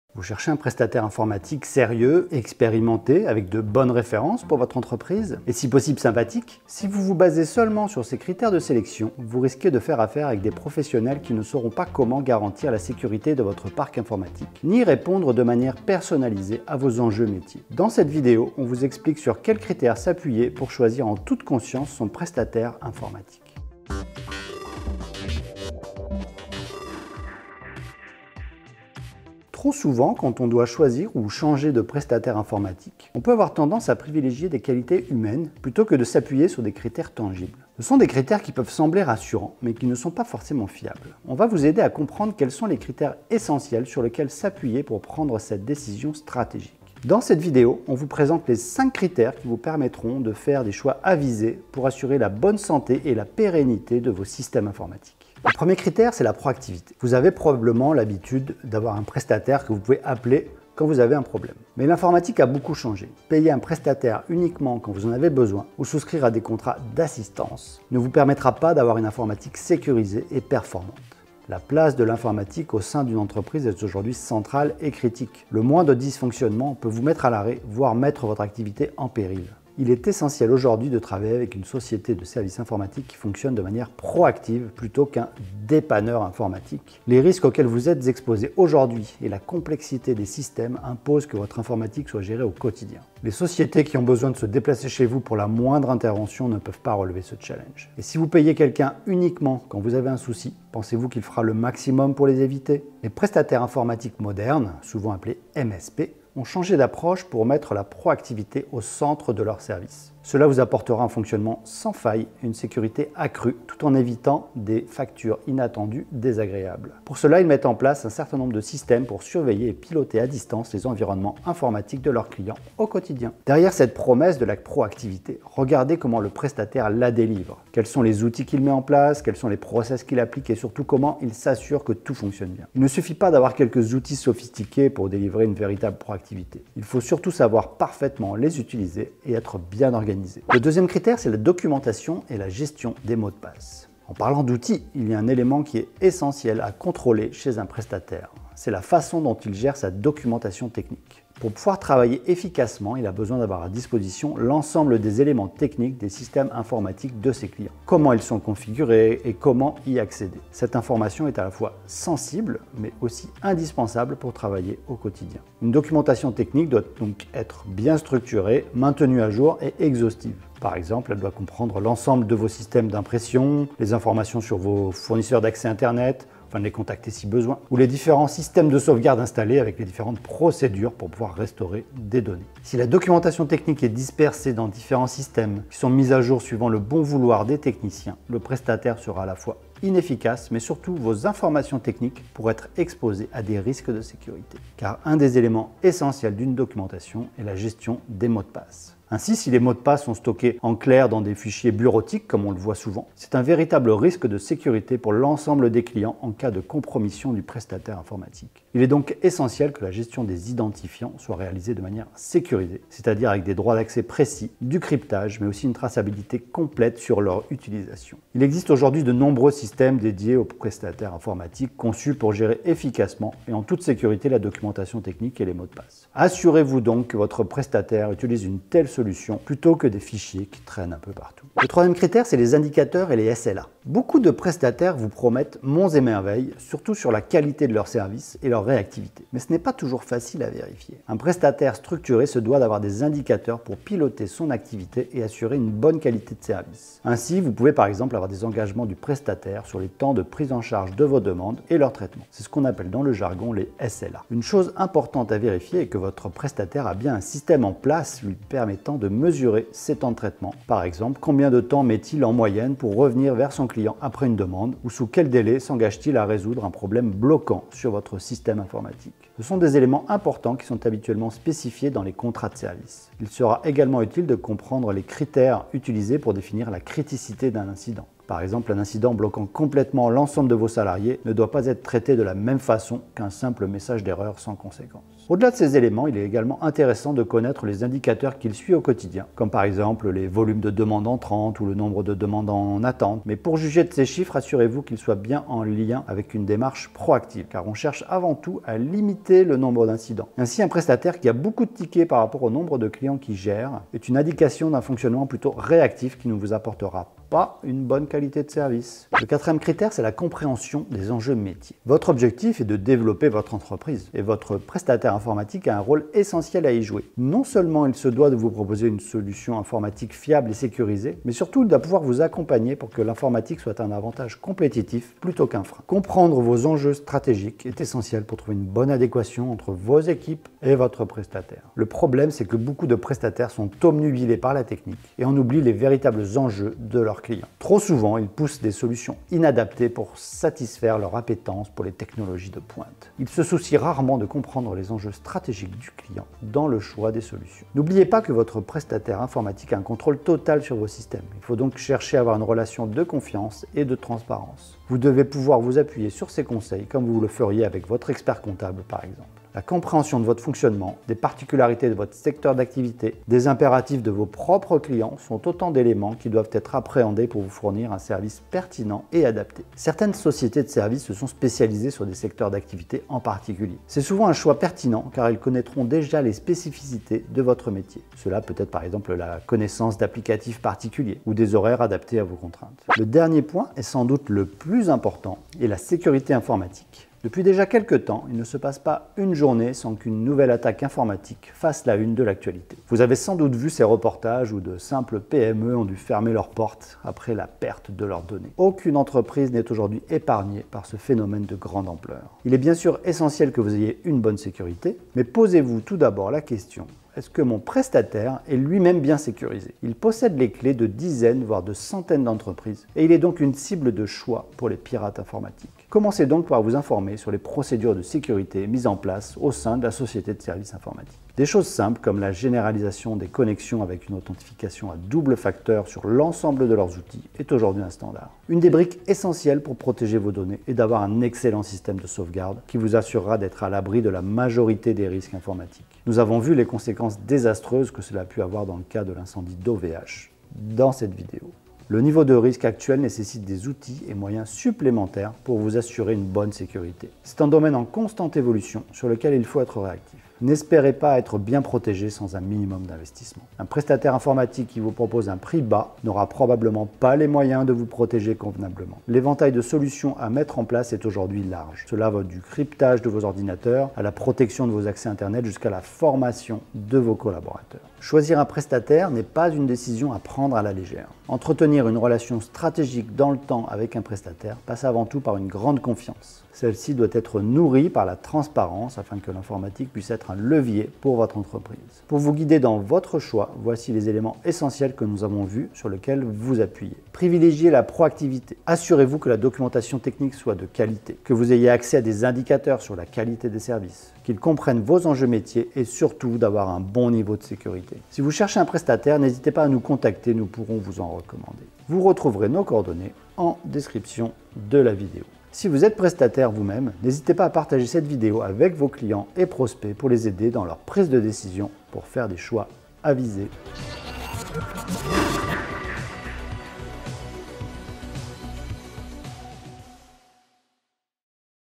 Choisir son prestataire informatique en toute confiance
Trop souvent, quand on doit choisir ou changer de prestataire informatique, on peut avoir tendance à privilégier des qualités humaines plutôt que de s’appuyer sur des critères tangibles.

Ce sont des critères qui peuvent sembler rassurants, mais qui ne sont pas forcément fiables.

On va vous aider à comprendre quels sont les critères essentiels sur lesquels s’appuyer pour prendre cette décision stratégique.

Dans cette vidéo, on vous présente les 5 critères qui vous permettront de faire des choix avisés pour assurer la bonne santé et la pérennité de vos systèmes informatiques.

Sommaire
00:00 Intro
0:23 Générique
0:55 La pro activité
2:20 La documentation et gestion des mots de passe
3:50 Les indicateurs et les SLA
5:08 Compréhension des enjeux métiers
6:25 La sécurité informatique
8:08 Conclusion